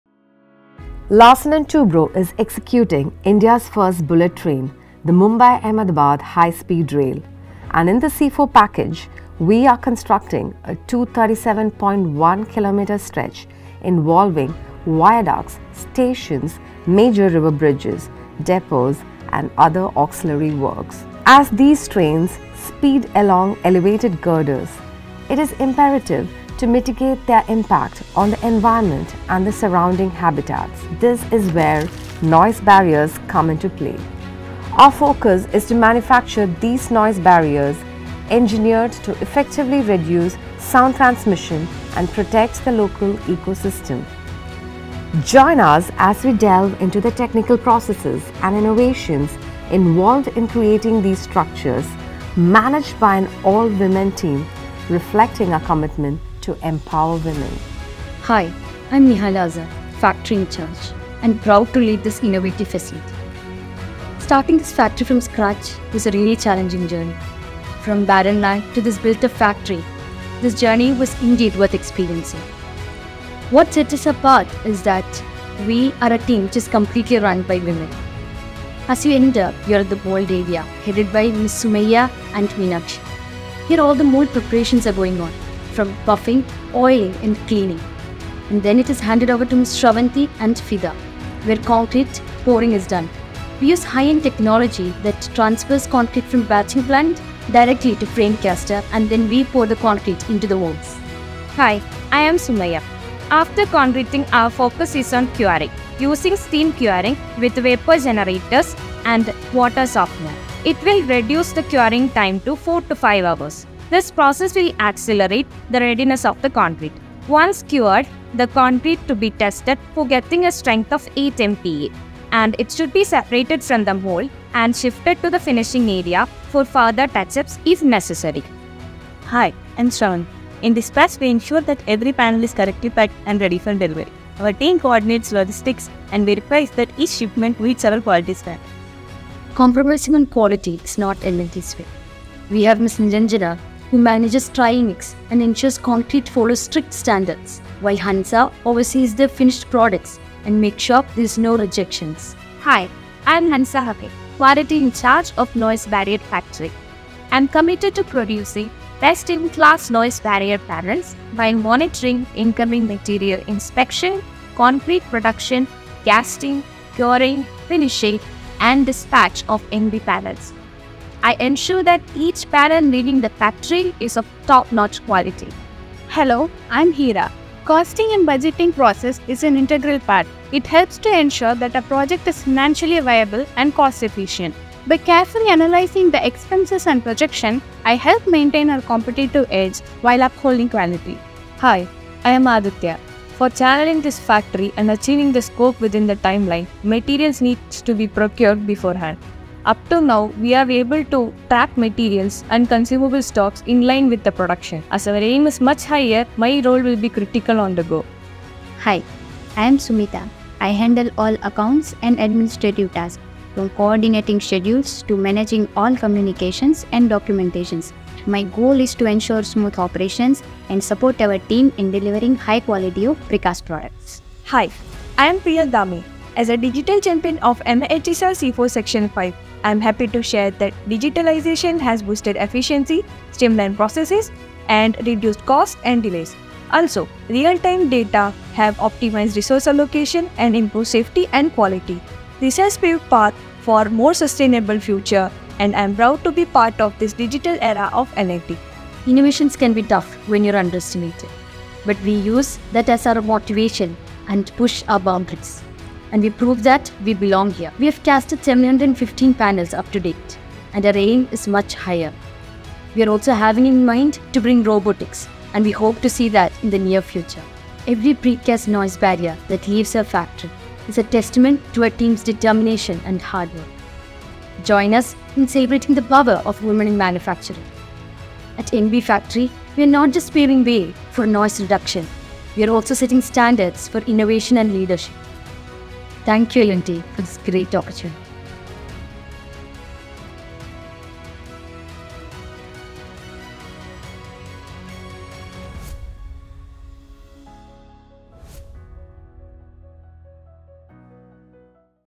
Empowering Women at the Mumbai-Ahmedabad High Speed Rail Project.
Discover the all-woman team that manages L&T’s Noise Barrier Factory at the MAHSR C4. From planning, budgeting and manufacturing the concrete barriers critical to this project, to quality testing and dispatch, this team is setting standards for innovation and leadership while contributing on one of India’s foremost infrastructure projects.